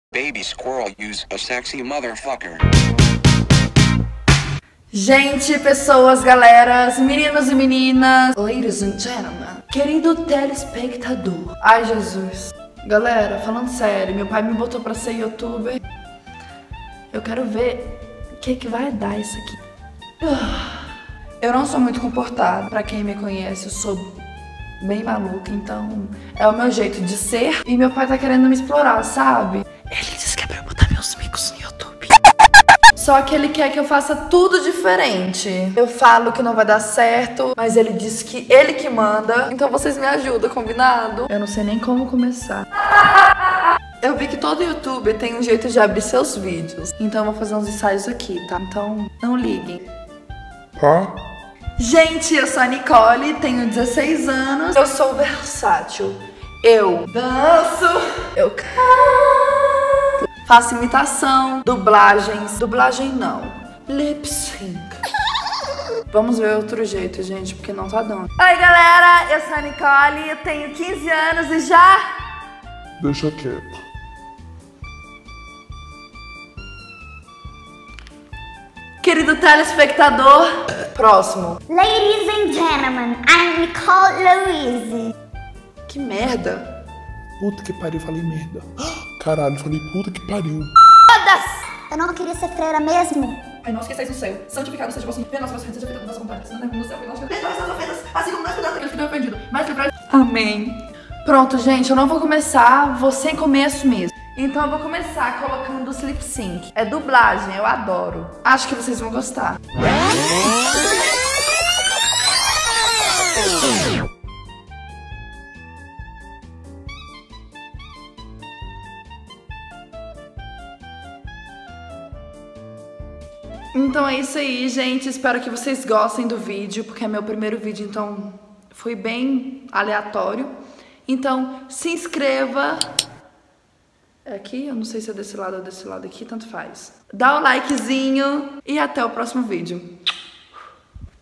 Nicole Louise - APRESENTAÇÃO DO CANAL
Oi galerinha!! Esse é o primeiro vídeo do meu canal e a continuidade vem depois!

Não se esqueça de opinar sobre o que você quer ver nos próximos vídeos!!